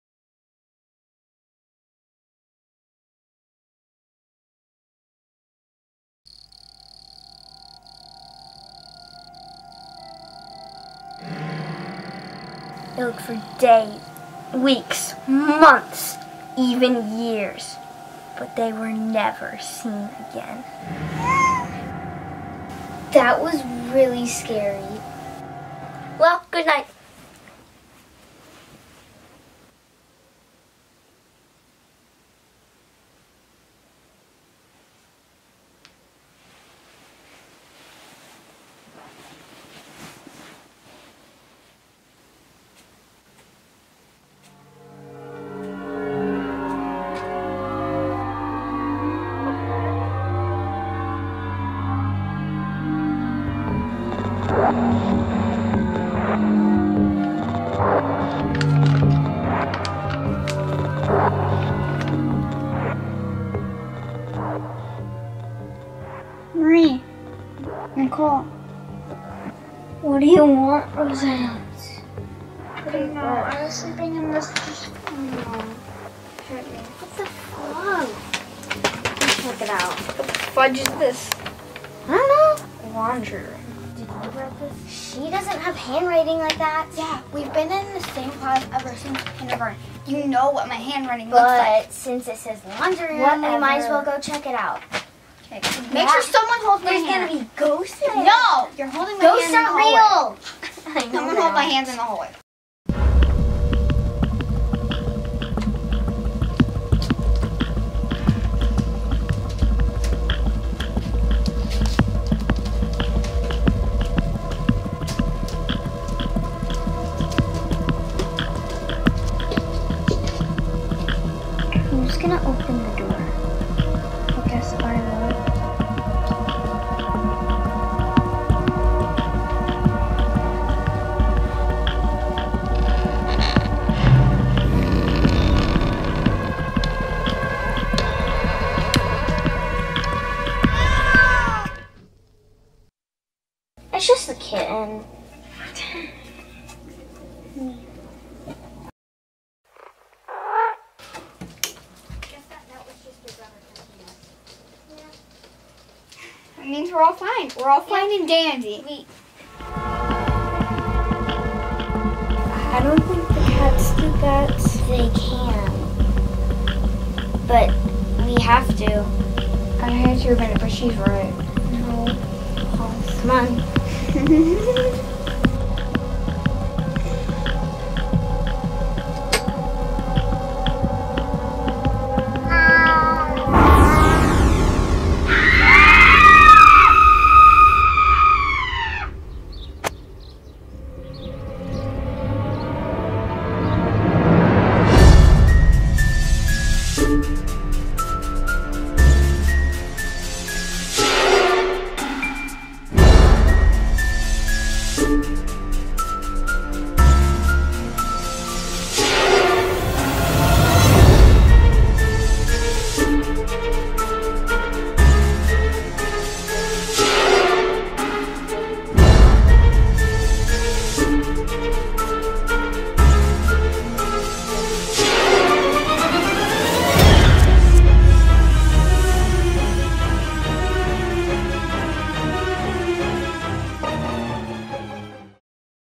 Killer Kitty | Short Horror Film for Kids | Warwolf Productions
A short horror film created by three 9-year-old girls.

Logline - Three girls encounter terror at a sleepover.